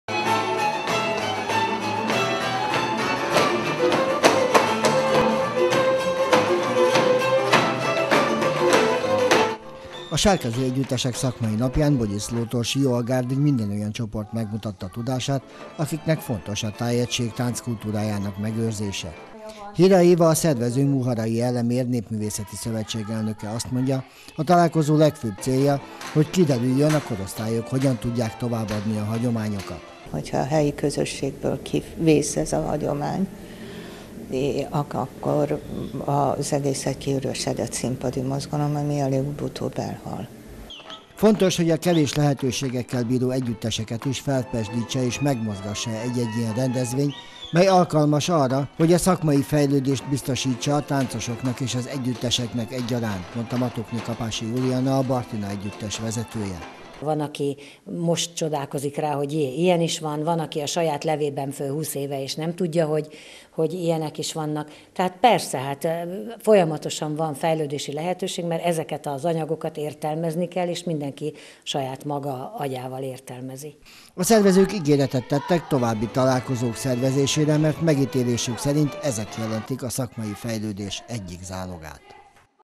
Sárközi találkozó - Bogyiszló 20191111
A Sárköz tánc kultúrájának legszebb elemeit mutatták meg a tájegységben működő együttesek Bogyiszlón. A Muharay Elemér Népművészeti Szövetség és a Bartina Néptánc Egyesület közös programjának célja, hogy a csoportoknak biztosítsa a fejlődés és a fellépés lehetőségét